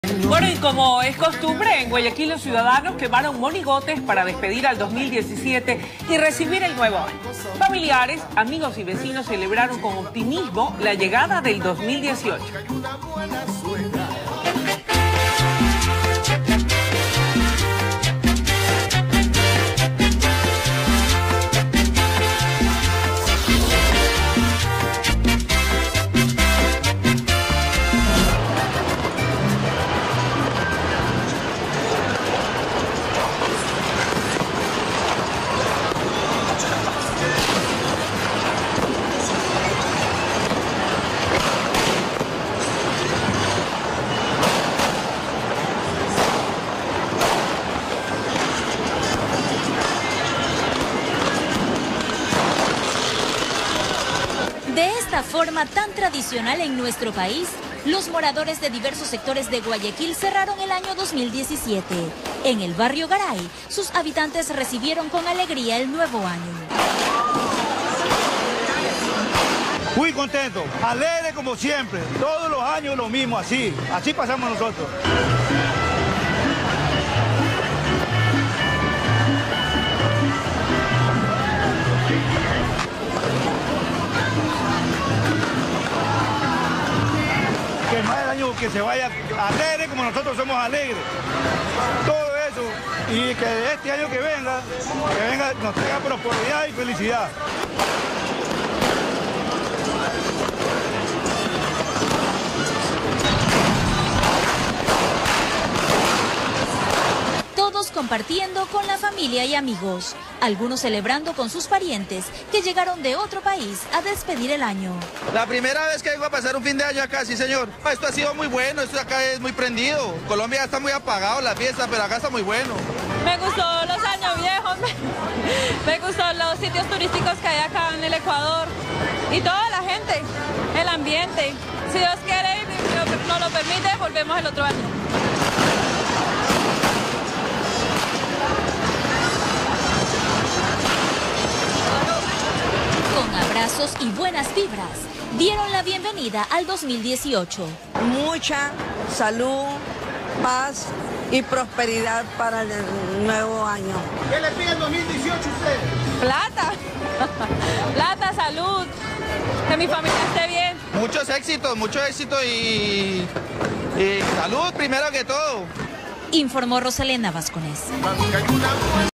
Ciudadanos despidieron el 2017 con la tradicional quema de los años viejos
Celebraron con optimismo y esperanza la llegada del año nuevo 2018

Todos en las calles junto a sus familiares se reunieron para efectuar esta tradición ecuatoriana.